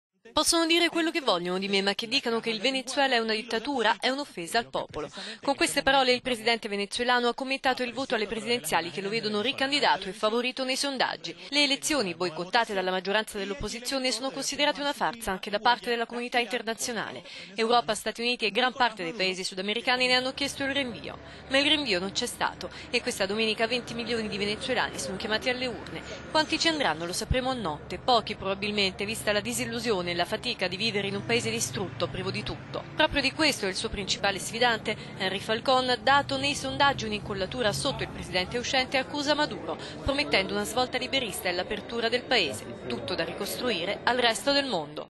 Venezuela al voto: boicottaggio dell'opposizione e astensionismo
Venti milioni di venezuelani chiamati alle urne. Maduro è insidiato da Henri Falcon che propone un programma liberista ma, secondo i sondaggi, l'astensionismo favorirà il presidente uscente. …